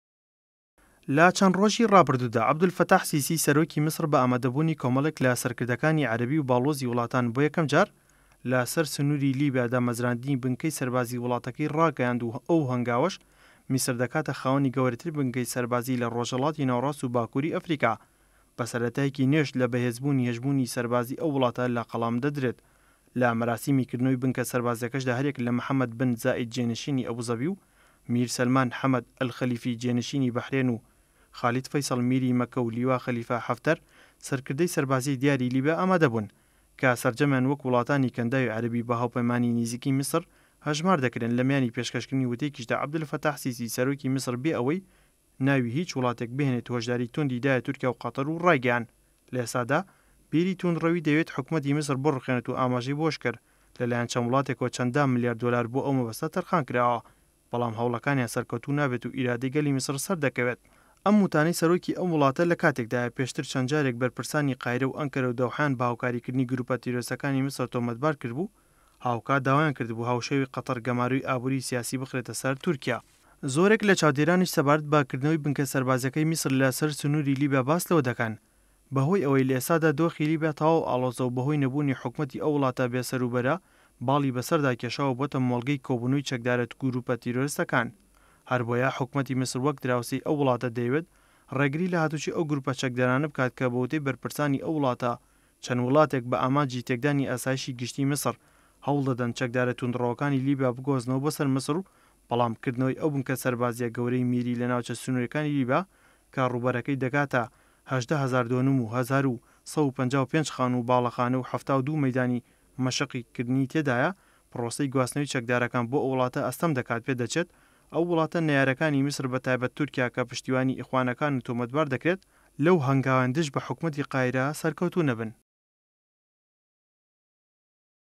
GK_ میسر - حكومەت گەورەترین بنكەی سەربازی لەسەر سنوری لیبیا دامەزراند
میسر - حكومەت گەورەترین بنكەی سەربازی لەسەر سنوری لیبیا دامەزراند